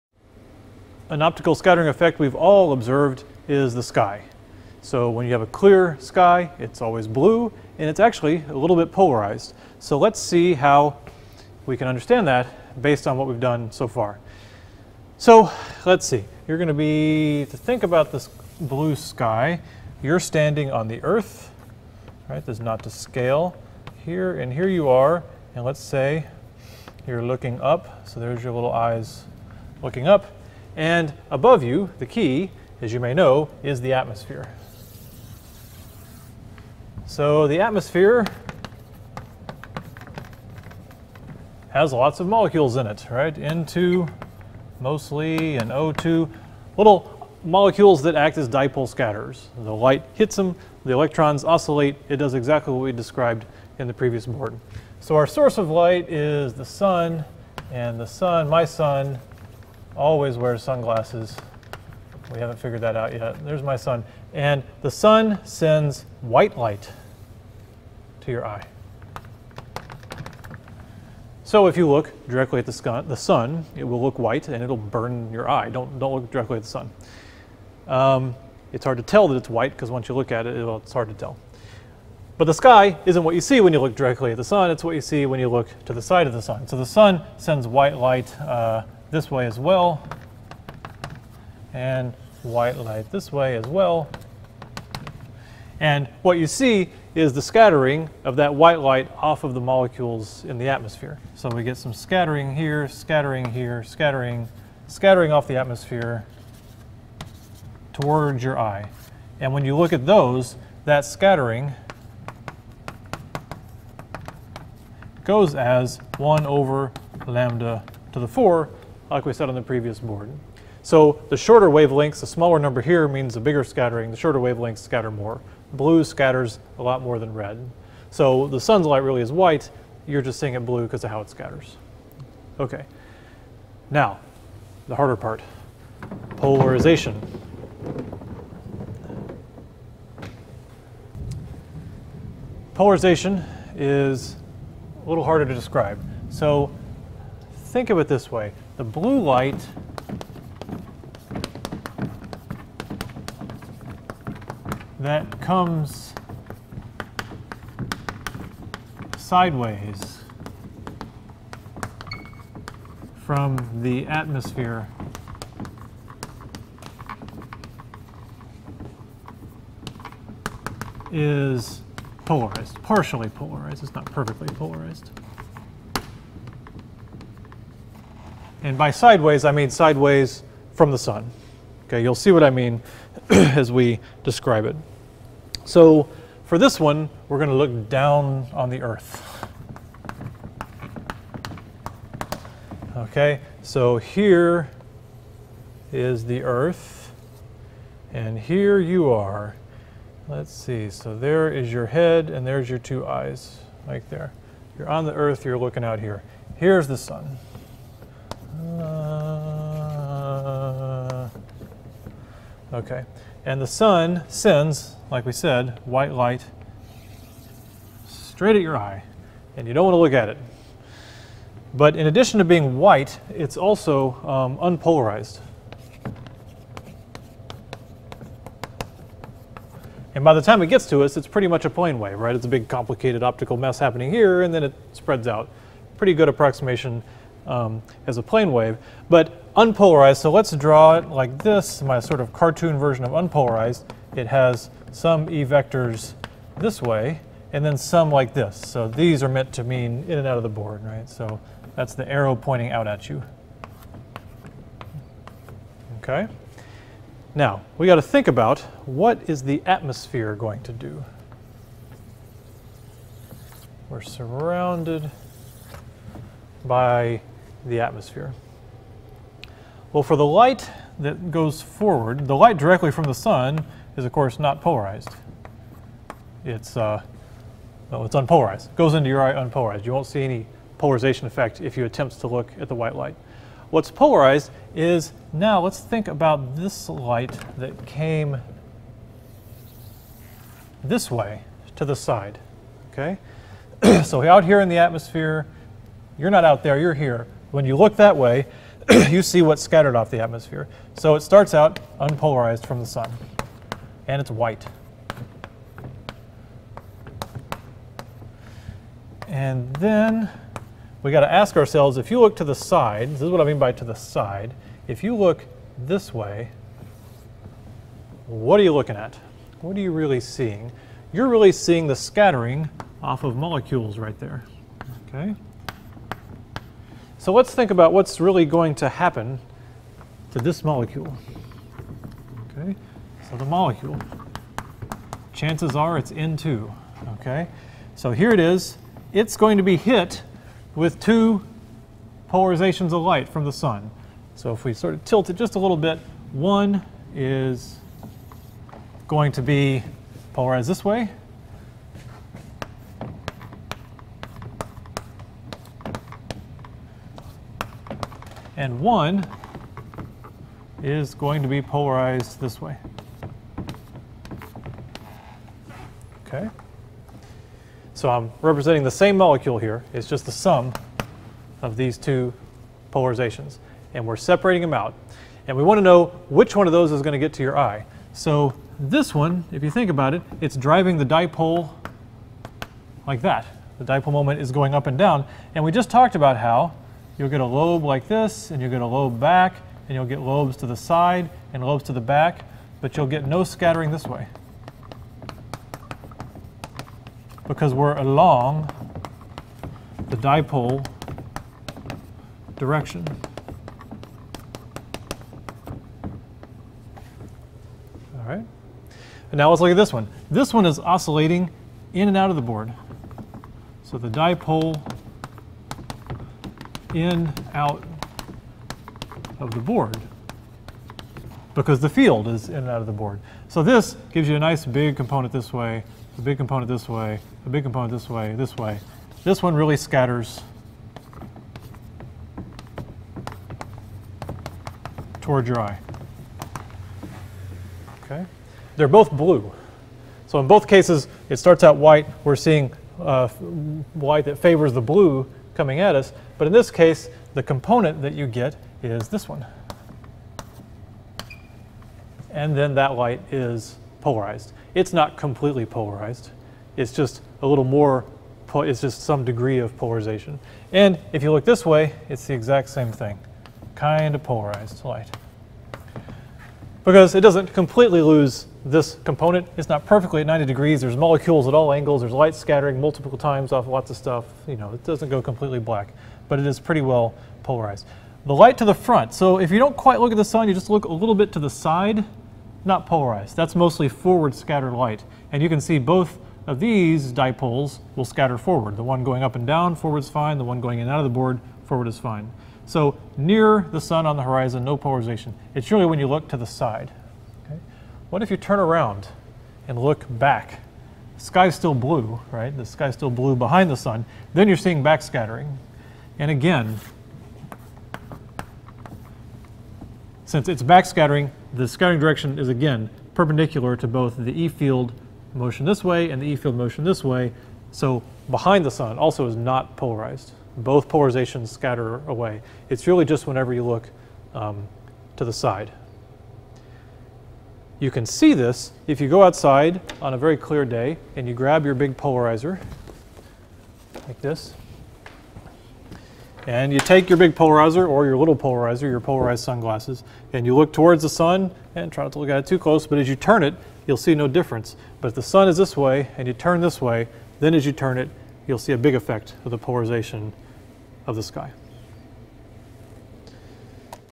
PHYS 201 | Polarized Scattering 2 - The Blue and Polarized Sky
Here we think about the geometry of how we see sunlight as it travels through and scatters off of the atmosphere.  We consider both the resulting spectral properties (why the sky is blue) and the sky's polarization.